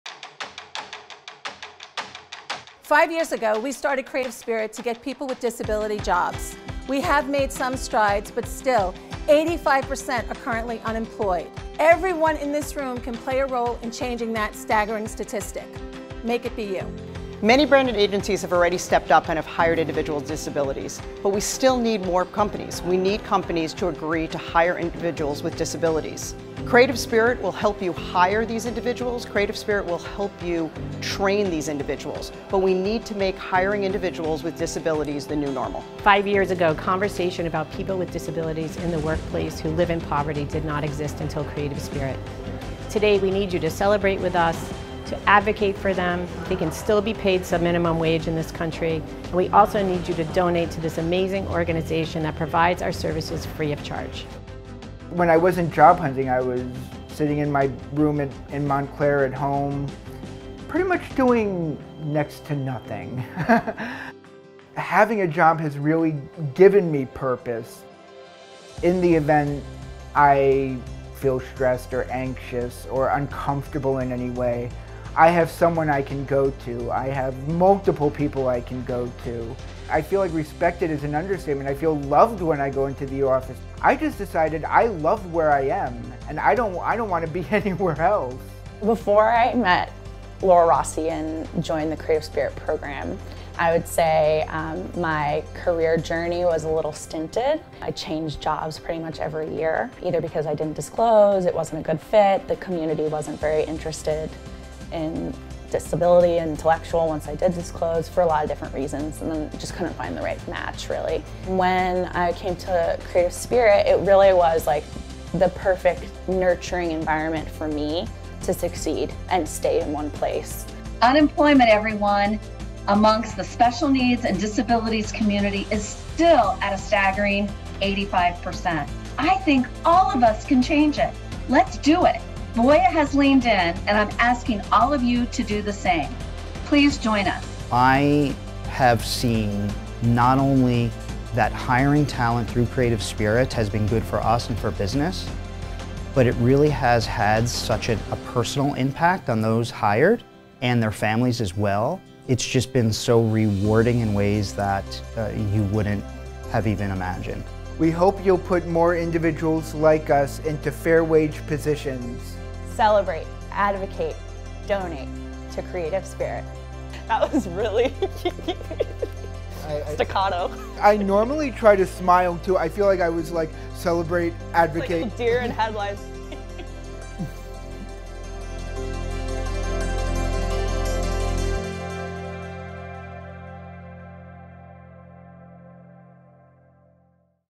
Creative Spirit x Advertising Week Birthday Bash Fundraiser
Five years have passed since the founding of Creative Spirit, and to celebrate, we’re sharing a video highlighting our fundraiser x birthday event that took place at Music Hall of Williamsburg on Oct. 20. The event spotlighted our HireDifferent plan to place at least 1 million candidates in fair-wage jobs by 2030. It featured a silent auction, stand-up comedy, and a live performance by Ludacris.